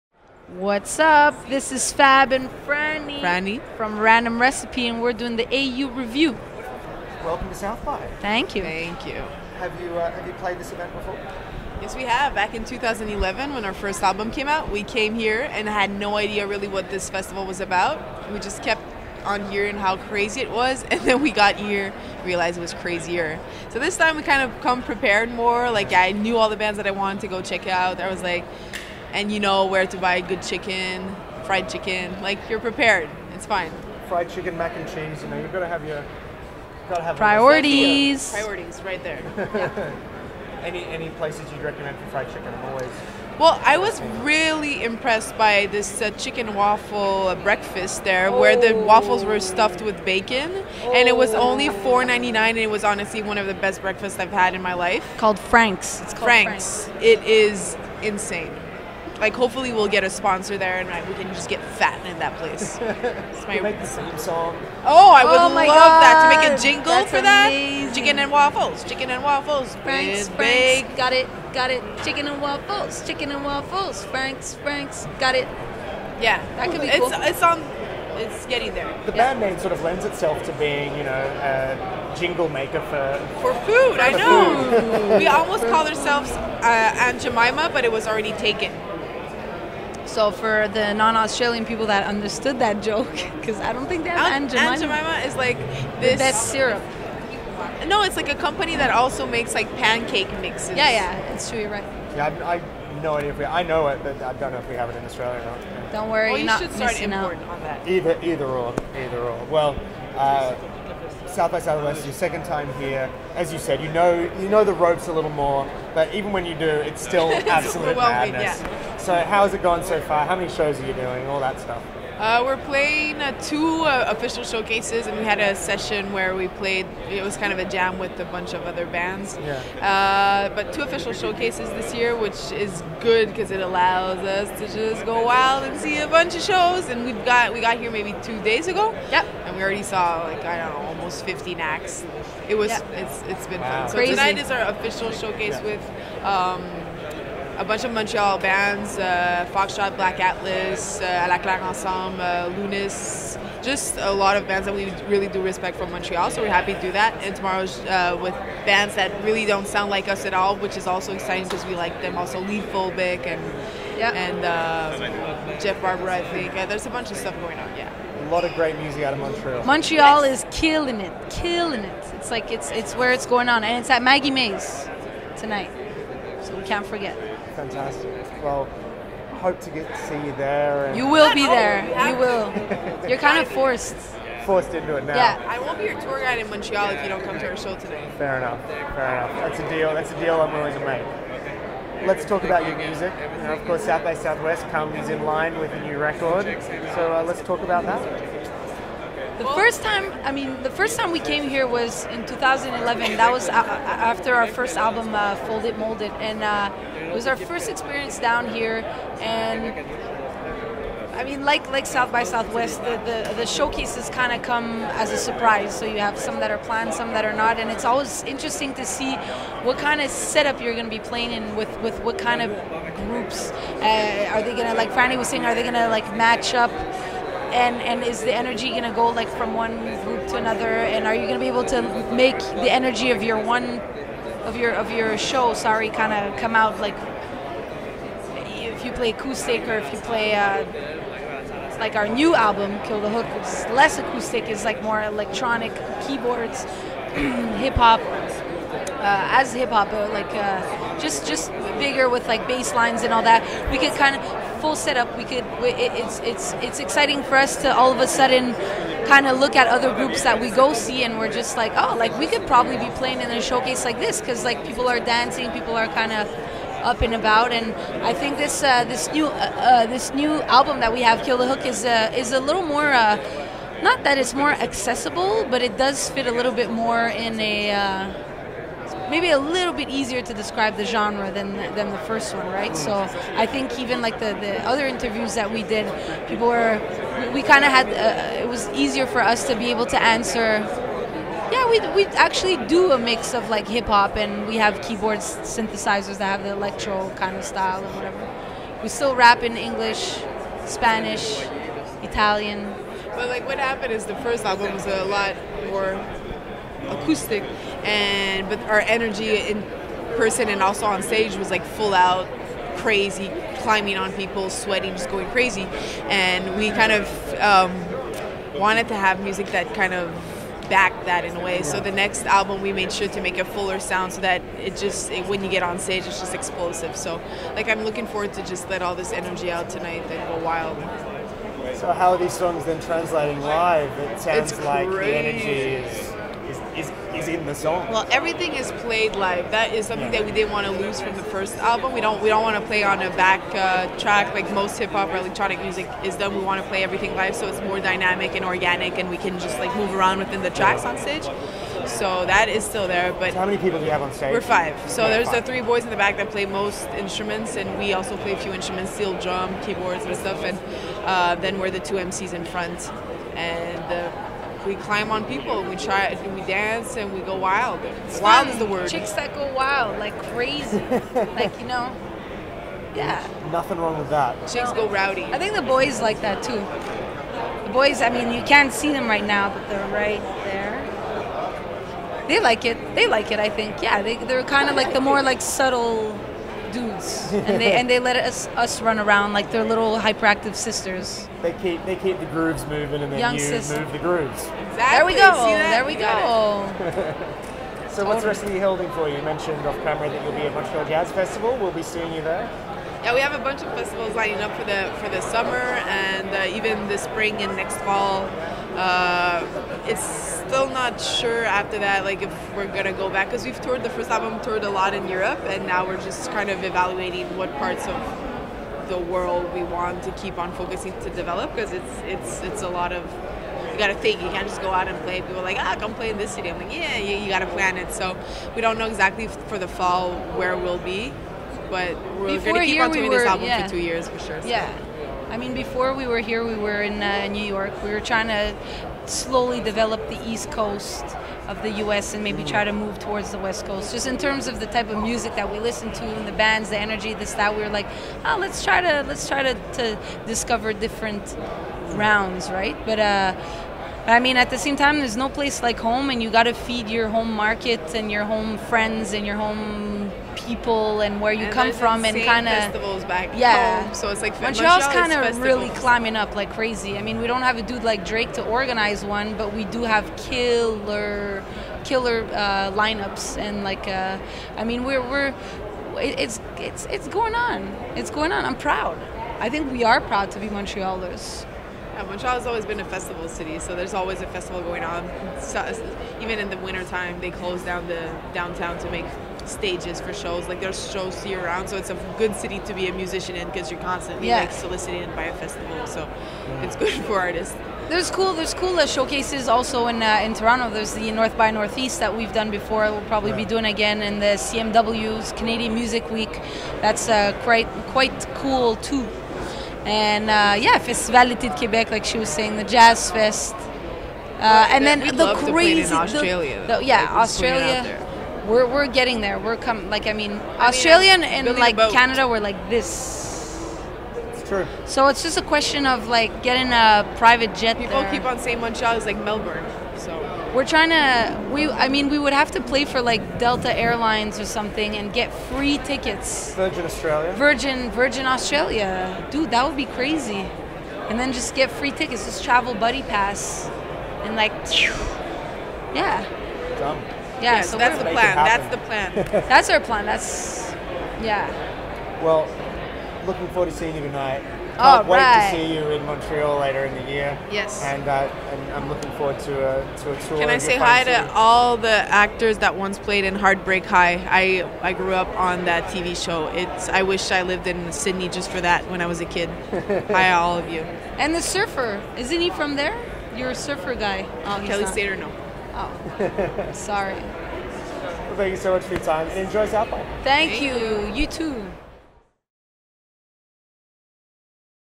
Interview: Random Recipe at SXSW 2014!
the AU review interviews Random Recipe at SXSW 2014.

Interview/Edit/Camera: Larry Heath

Filmed on site at SXSW in Austin, TX - March 2014.